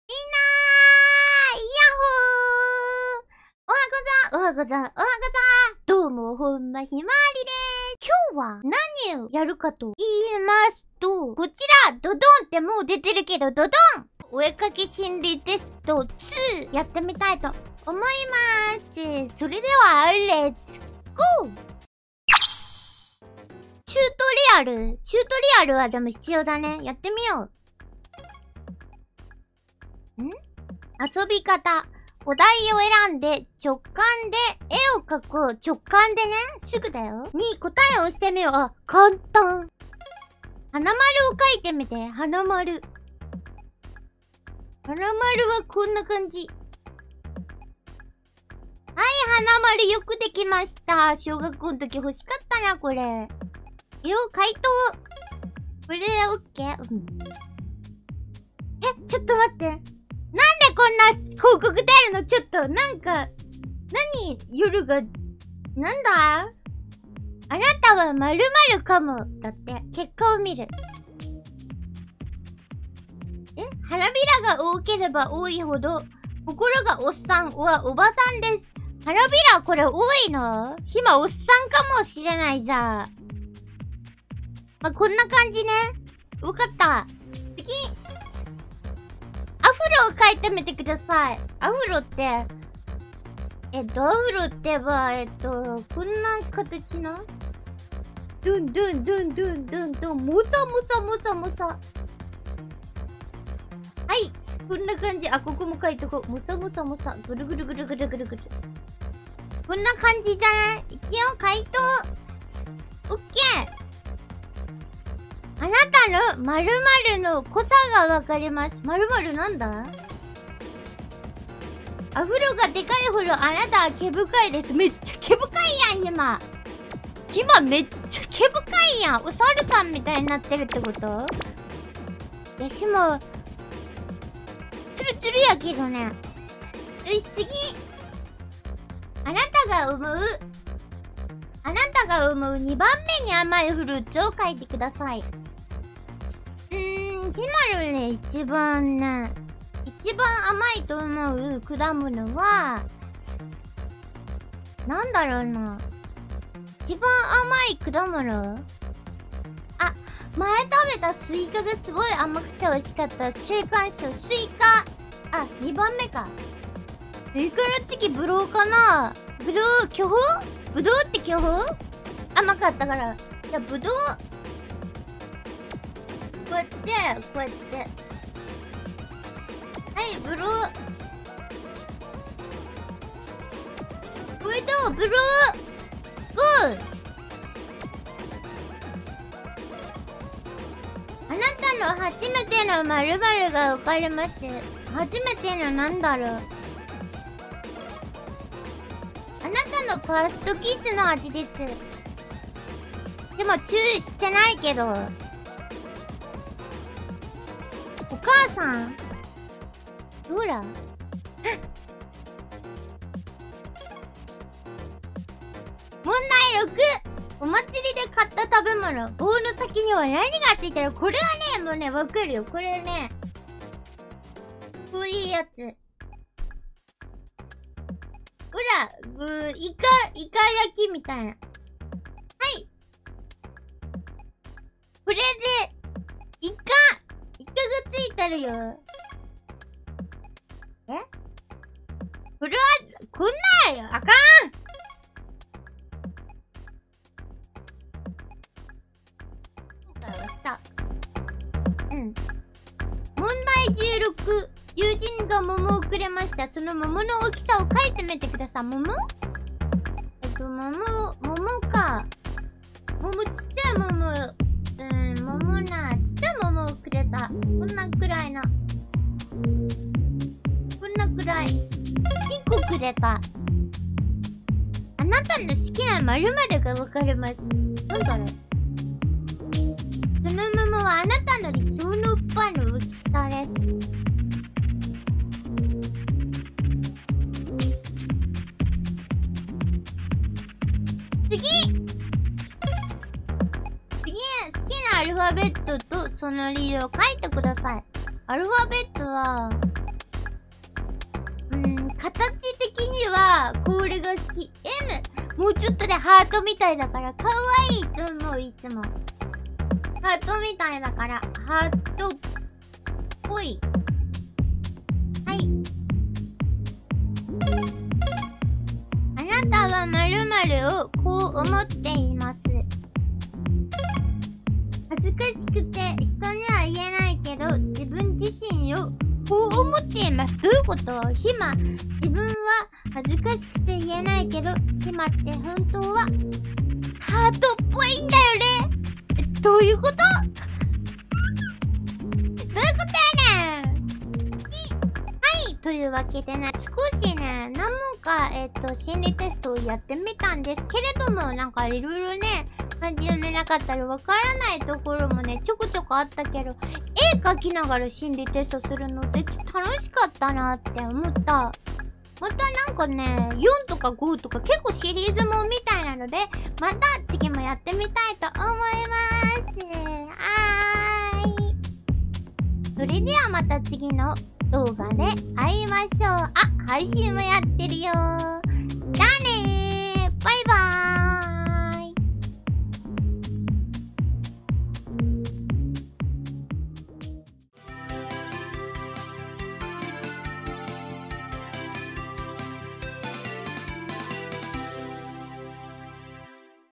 【お絵かき心理テスト】丸わかり！こんなのＮＧワードだぁ！【本間ひまわり】
おはござ～(・ワ・🌻)ﾉ
Vtuber本間ひまわりです！

今回はリスナーさんからもオススメして頂いた
心理テスト通というアプリをプレイしてみたよ！

ちょっと変な質問多くてびっくりしたよ！

ʚ Youtube ɞ
本間ひまわり - Himawari Honma -